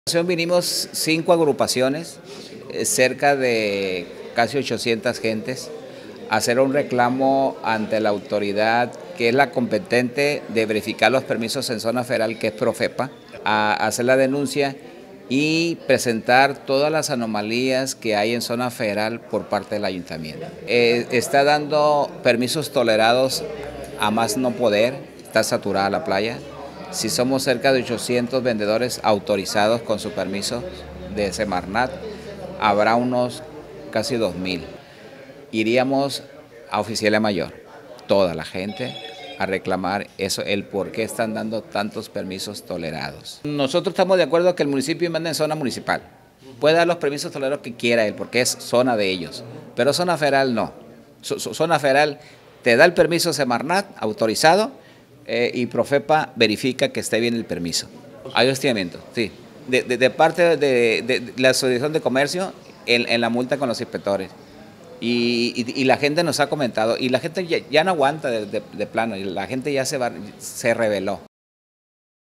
Acusan al Gobierno del Químico de Otorgar Más de Mil Permisos a “Tolerados”
*Alrededor de dos mil en zona de playa
*Oficialía los hostiga y retira mercancía